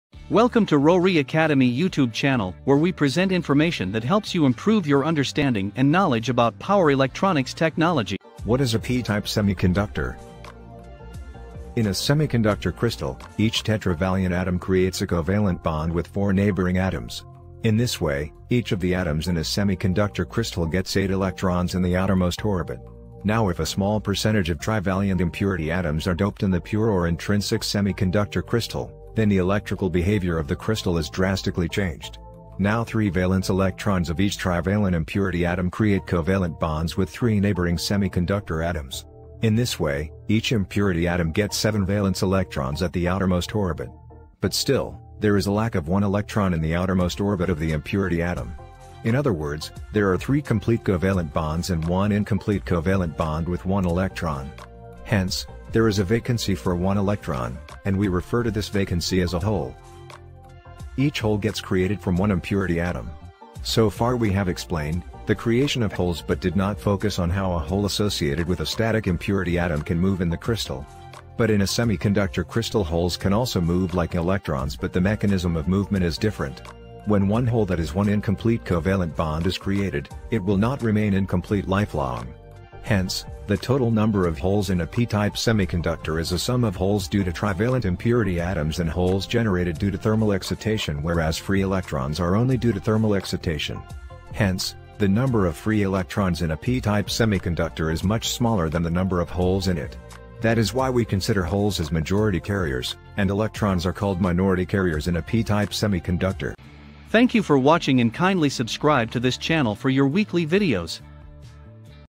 Understanding Power Diodes (Part 2): P-Type Semiconductor
This video explains the p-type semiconductor material in a signal diode.